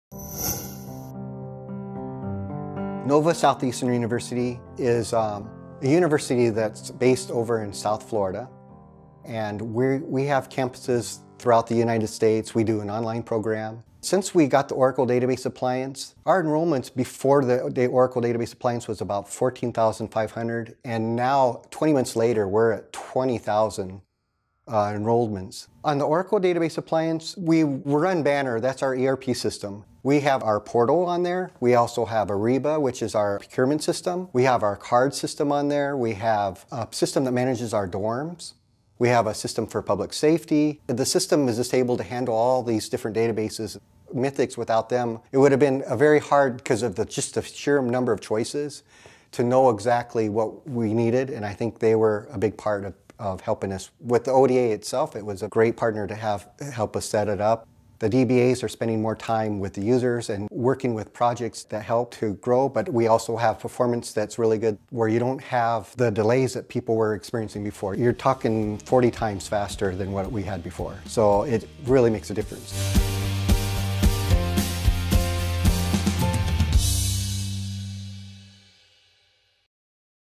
Nova Southeastern Gains 40X Faster Performance on ODA
Nova Southeastern University runs Banner with other applications on Oracle Database Appliance, with a performance improvement which resulted in a better user experience.